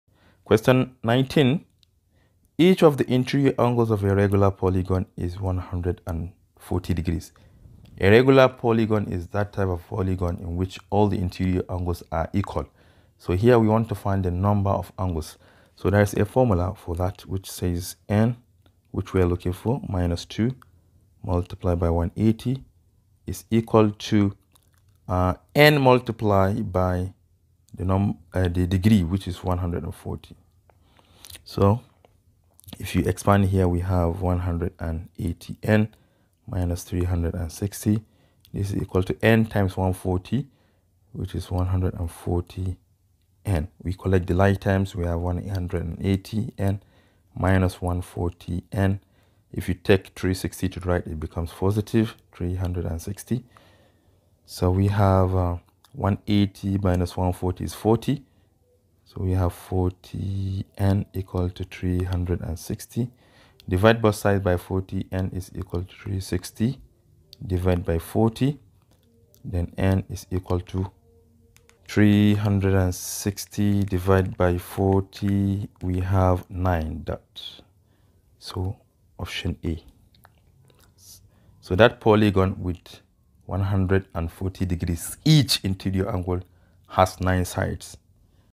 2021 JAMB Question 19 | How to find the number of sides of a regular polygon
Joint Admissions and Matriculation Board